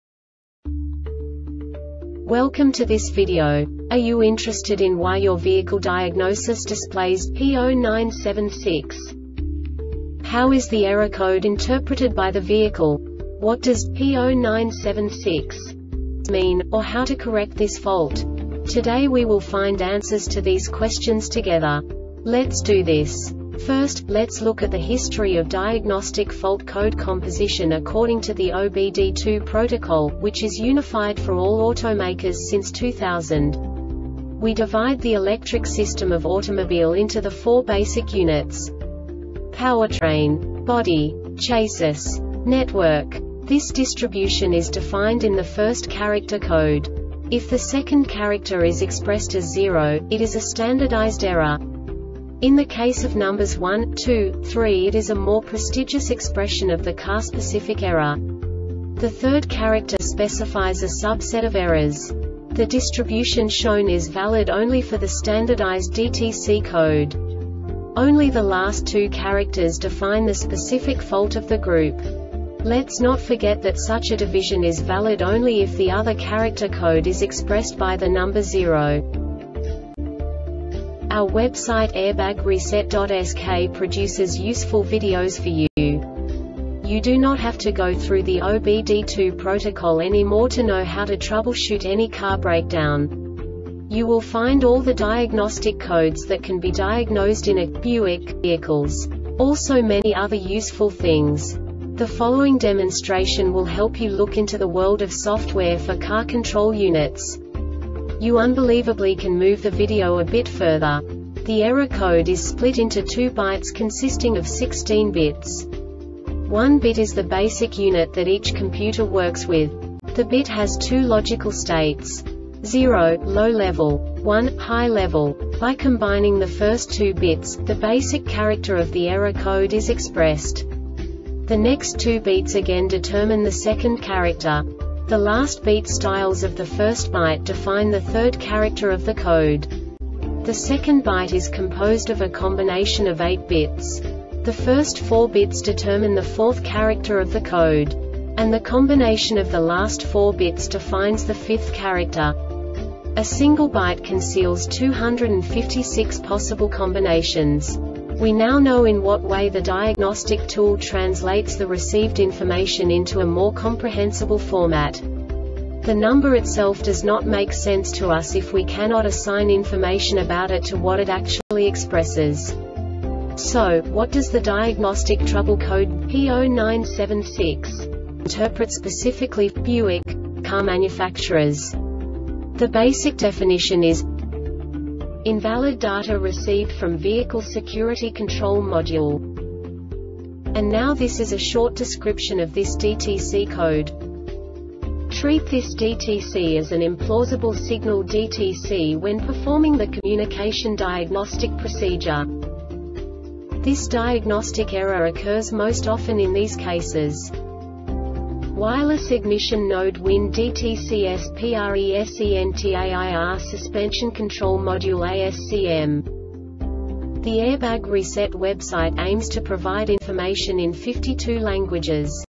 DTC Buick P0976 Short Explanation
Contents:
0:21 Basic DTC analysis according to OBD2 protocol standard.
1:48 Insight into programming
2:58 A diagnostically understandable description of the Diagnostic Trouble Code
3:05 Basic error definition

Make:
Buick
Code:
P0976
Definition:
Invalid Data Received From Vehicle Security Control Module
Description:
Treat this DTC as an IMPLAUSIBLE SIGNAL DTC when performing the COMMUNICATION DIAGNOSTIC PROCEDURE.
Cause:
WIRELESS IGNITION NODE (WIN) DTCS PRESENTAIR SUSPENSION CONTROL MODULE (ASCM)